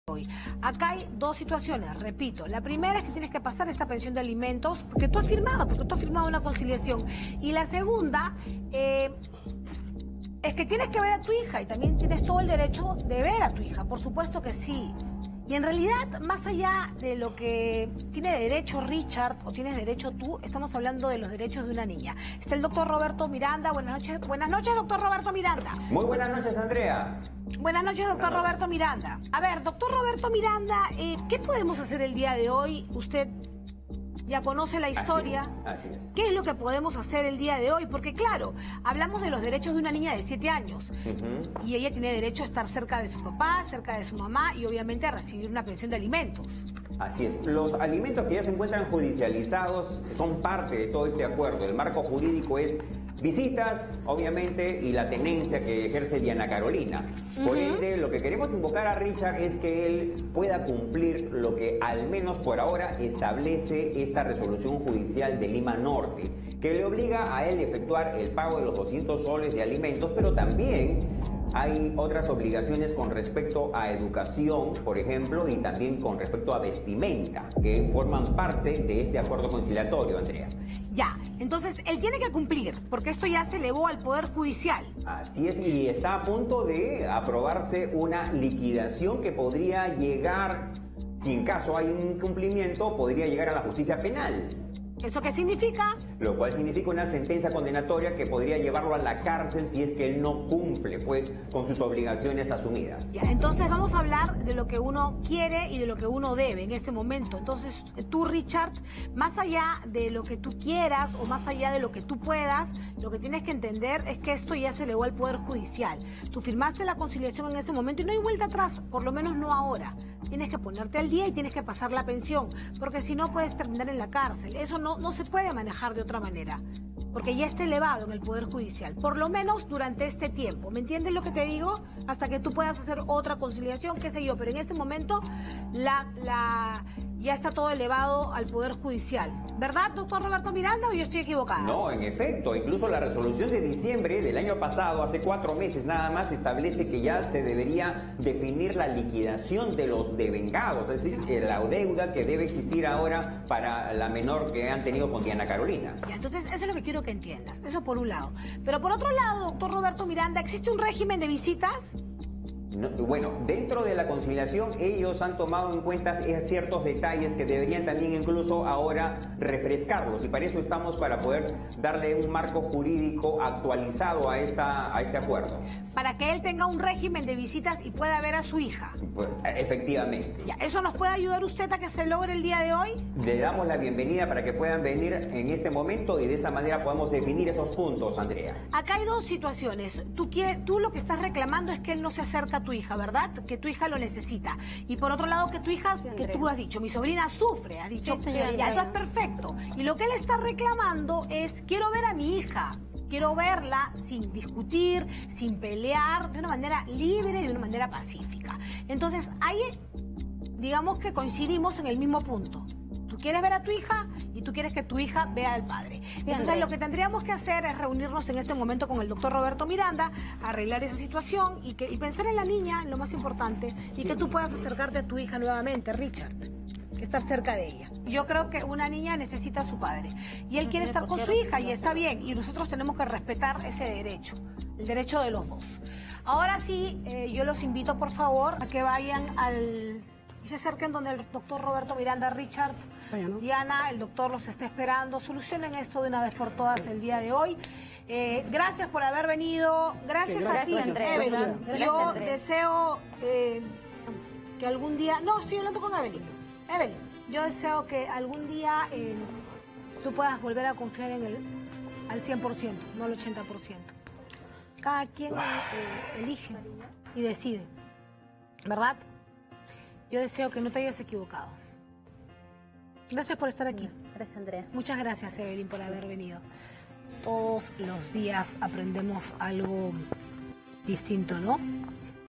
Tras una tensa discusión, Diana y Richard llegan a un acuerdo por el bienestar de su hija
Diana vino al programa para buscar justicia por su hija. Dice que él no se acerca a la niña pero él quiere verla sin problemas. Finalmente, ambos llegaron a un acuerdo por el bienestar de su hija.